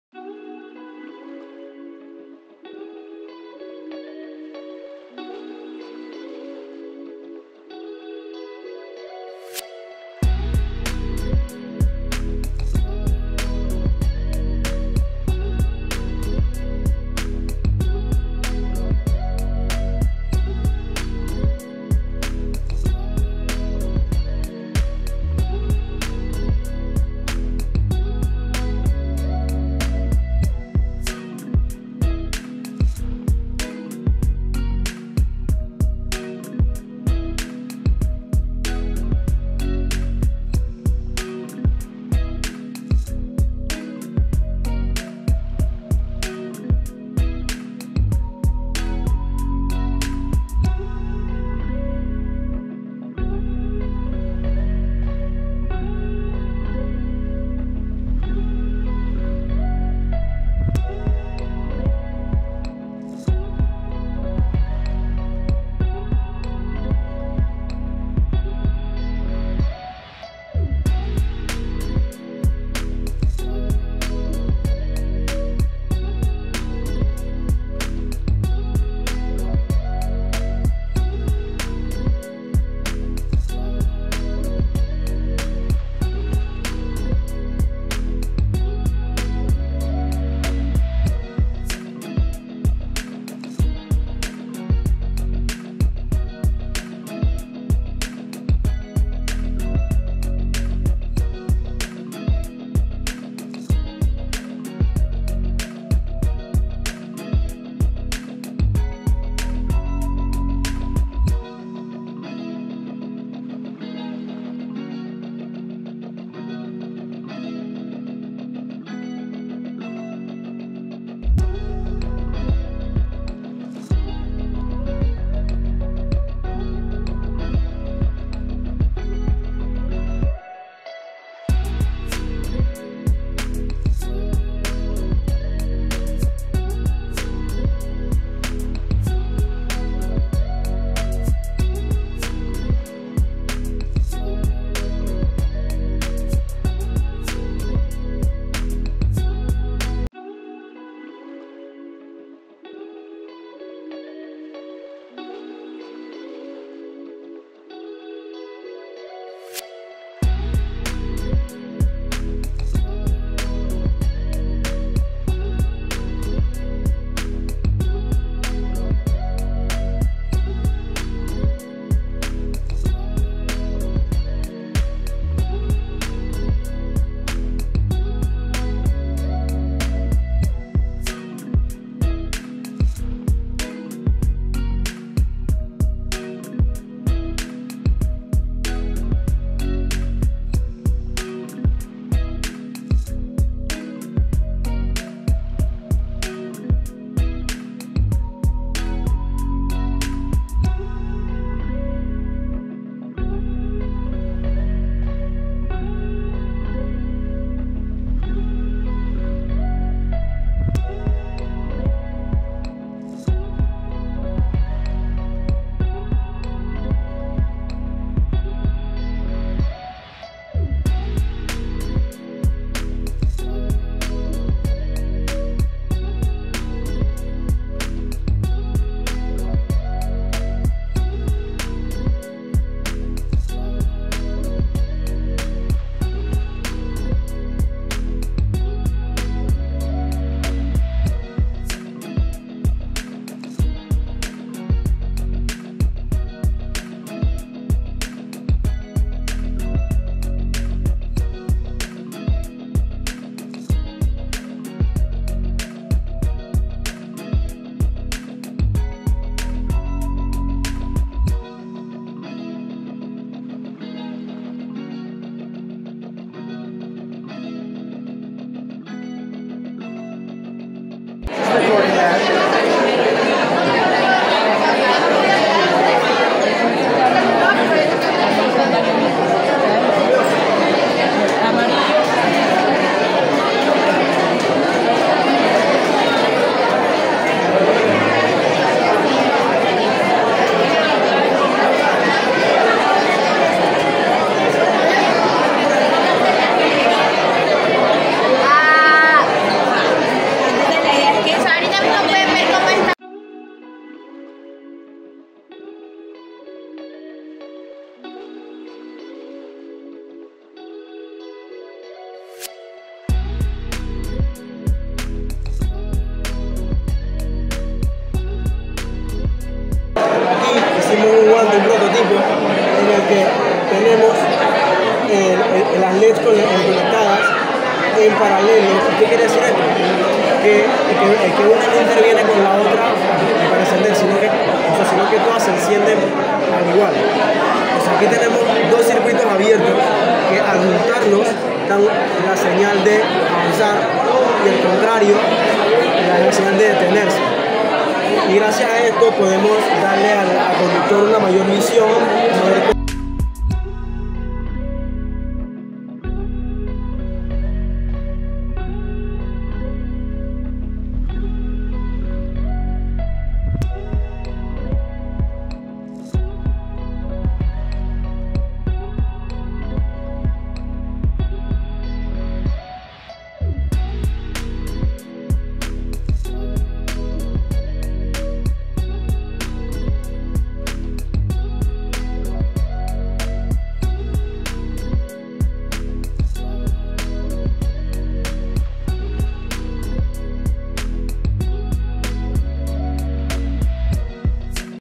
Feria Escolar de CIENCIA 💥WOW💥
A continuación de doy un recorrido por la feria escolar de proyectos y experimentos científicos.